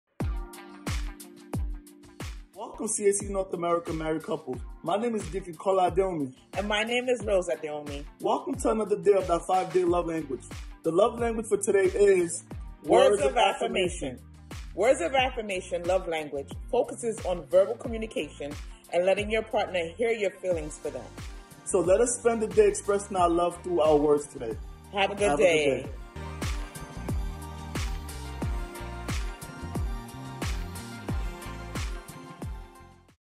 5 Day Love Challenge
Day #2 of our 5 Day Love Challenge. Today's focus is Words Of Affirmation.